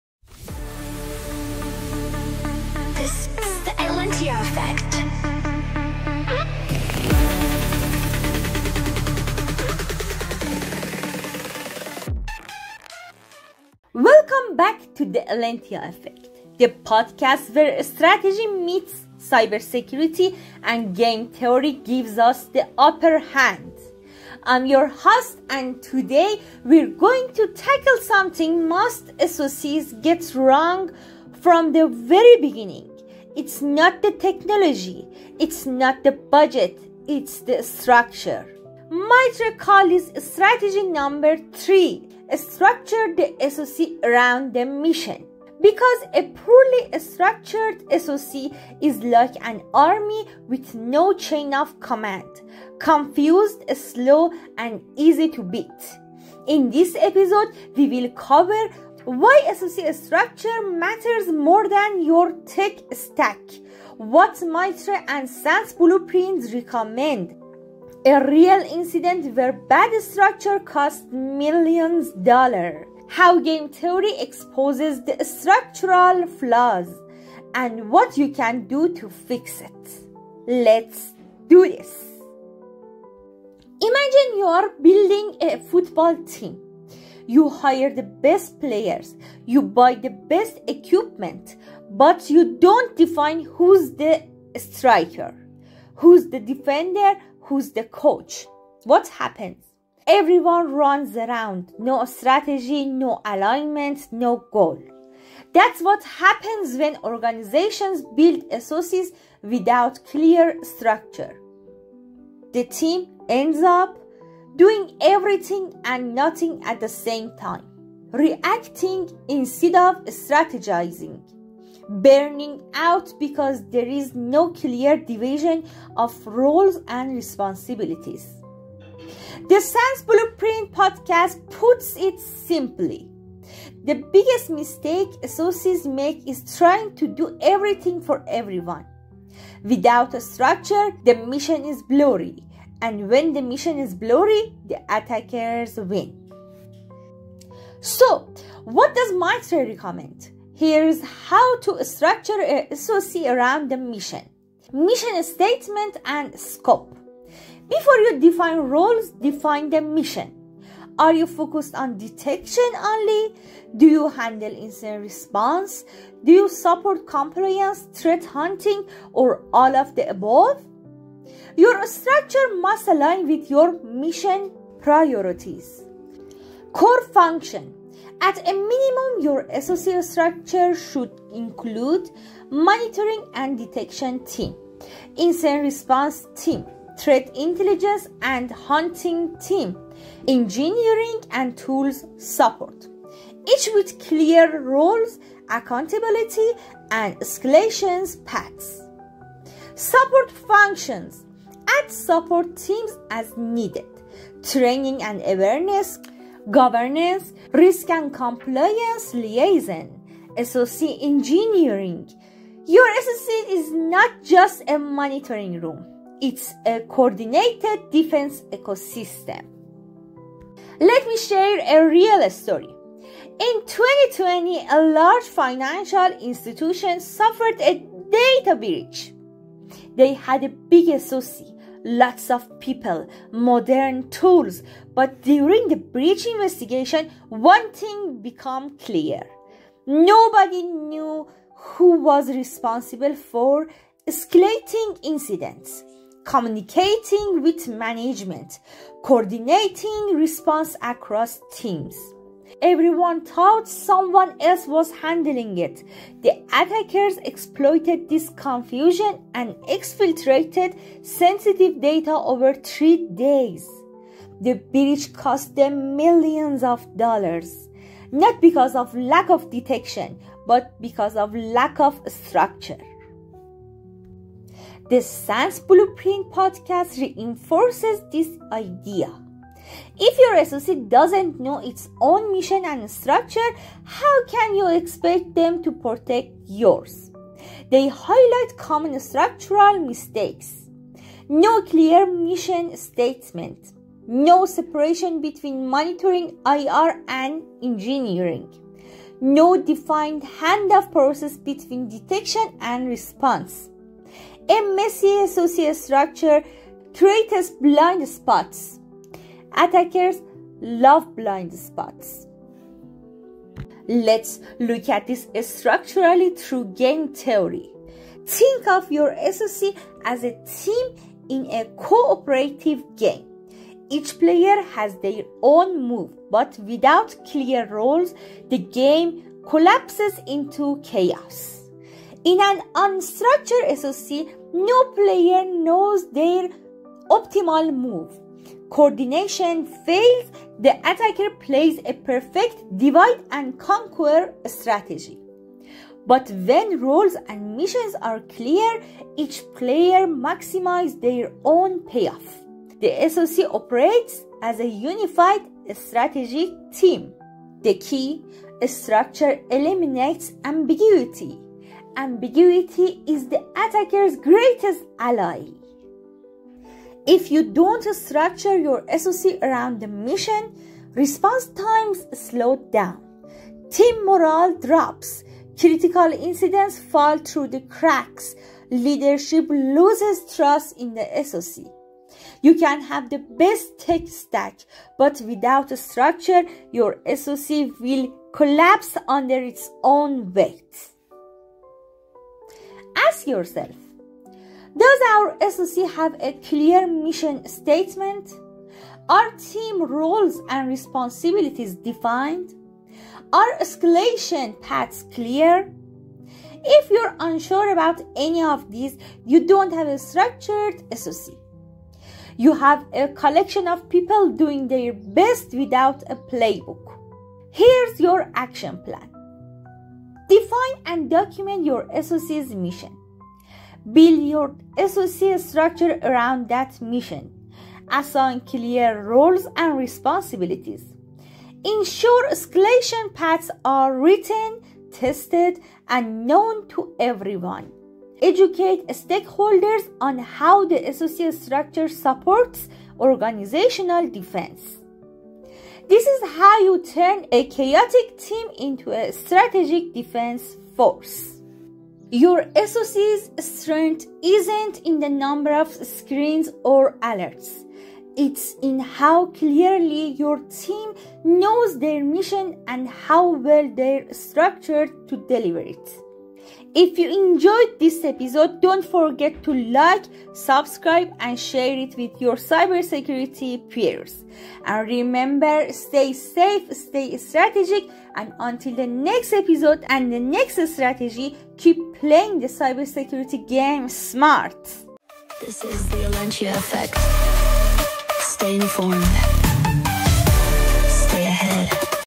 Blueprint or Breakdown? Structuring the SOC Around the Mission
In this episode of The Elentiya Effect, we explore MITRE SOC Strategy #3: Structure the SOC Around the Mission — and why so many SOCs fail not because of bad technology, but because of bad structure.

 Learn how a misaligned SOC leads to confusion, blind spots, and slow incident response — and how to fix it by building your SOC like a strategic team, not just a collection of tools.

 This episode covers:

    How to define a clear SOC mission

    What MITRE and the SANS Blueprint say about structure

    The core and support functions every SOC must have

    A real-world breach where structure failure led to disaster

    A game theory perspective on SOC coordination and payoff

    Actionable steps to restructure your SOC today

 Whether you're starting a new SOC or improving an existing one — this episode will give you the blueprint to build it around what matters: your mission.

 Tune in, take notes, and lead your SOC like a pro.